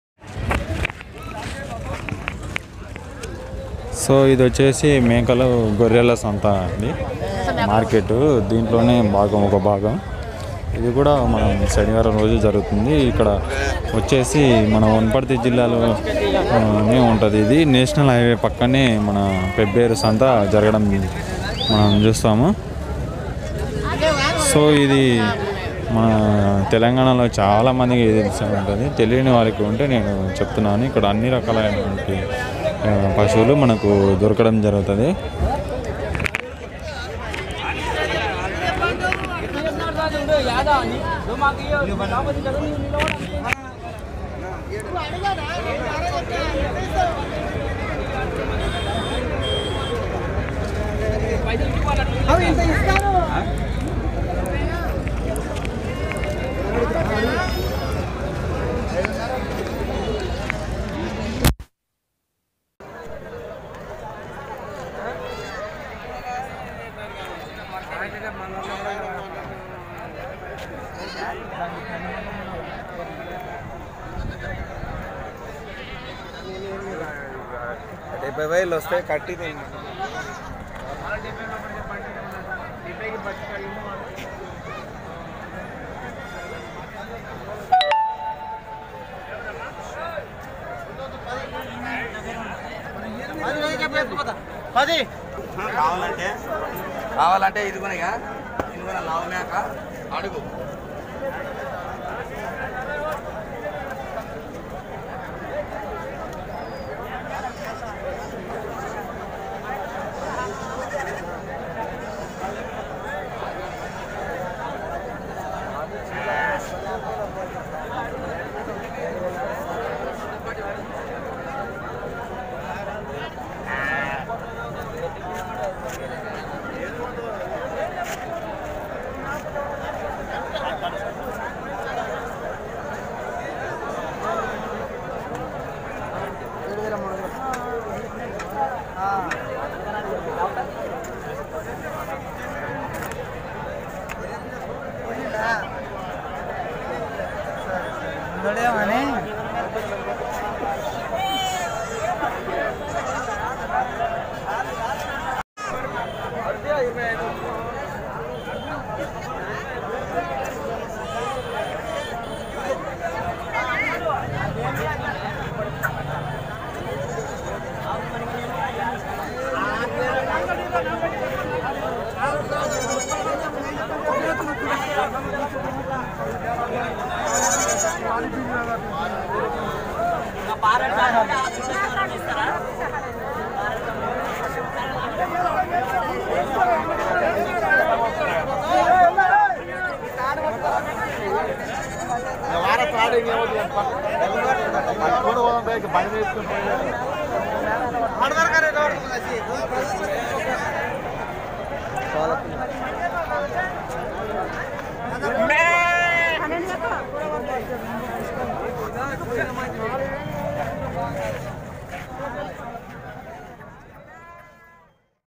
ప్రతి శనివారం పెబ్బేరు మేకల గొర్రెల సంత /! Saturday market in Pebbeir
ప్రతి శనివారం వనపర్తి జిల్లాలోని పెబ్బేరు సంత జరుగుతుంది మేకలు గొర్రెలు పశువులు గేదెలు కోళ్ళు అన్ని రకాల పక్షులు జంతువులు అమ్మబడును